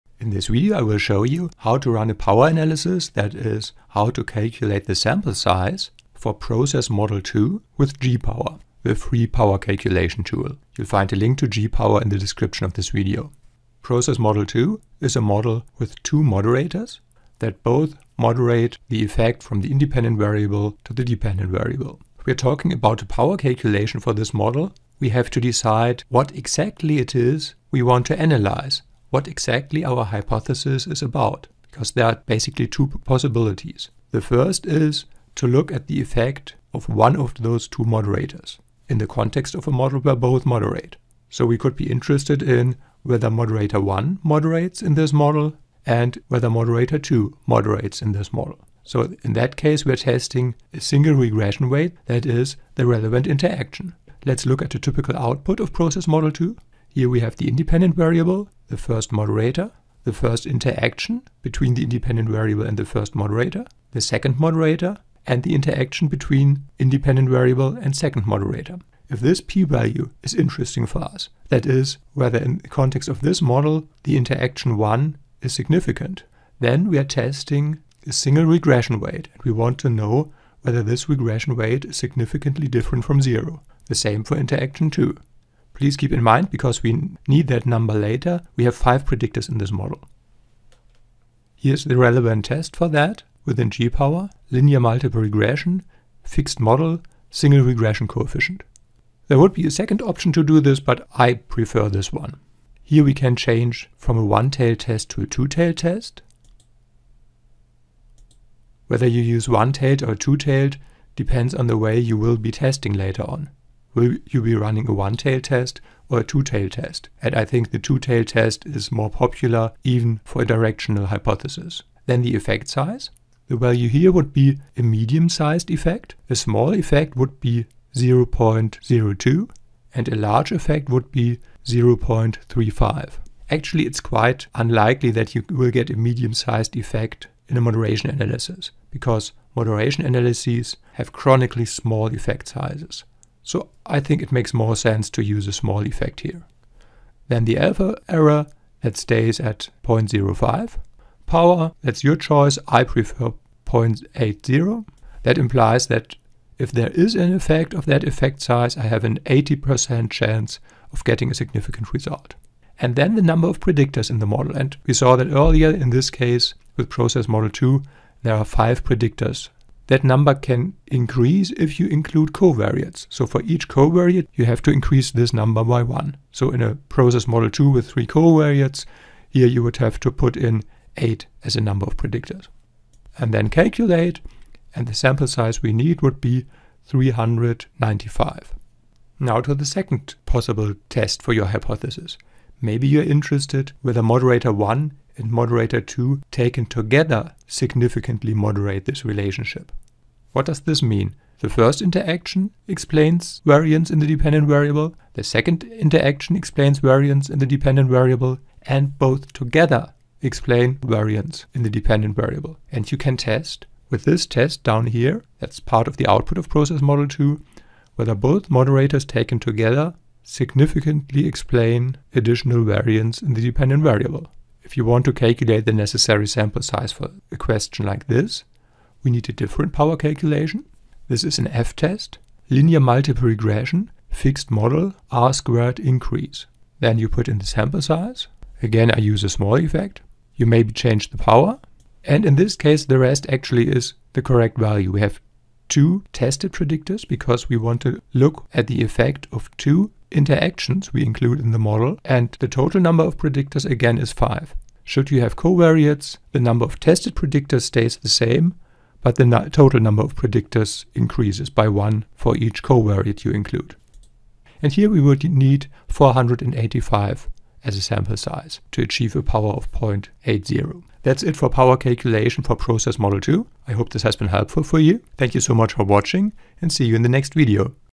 Power PROCESS Model 2 (Sample Size for Double Moderation)
If you want to use Hayes' PROCESS macro (with SPSS or with R) to test a double moderation (= model template 2), then you can calculate the necessary sample size with the free power calculation tool G*Power. This tutorial shows you how to do that.